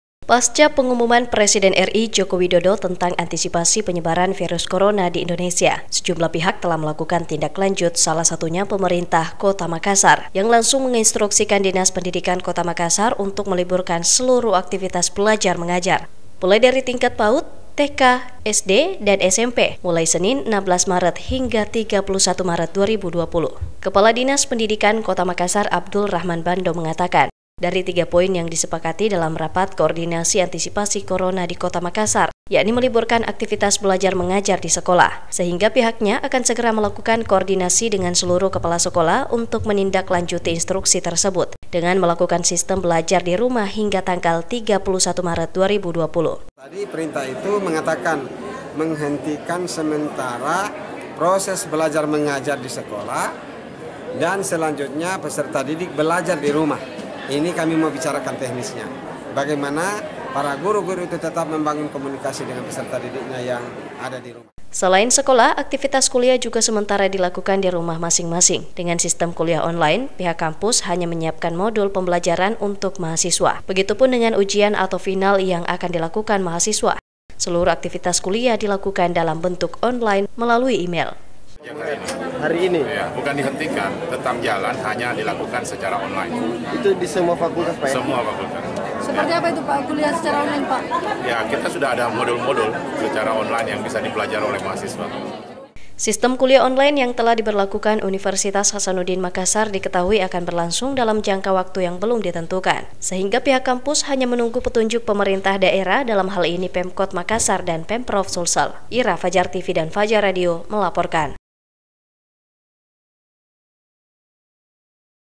Antisipasi Covid-19, Sekolah di Makassar Diliburkan hingga 31 Maret
Berita-Berita Fajar TV bisa disaksikan di Chanel 49 dan di Nonton di Streaming JPM Stream.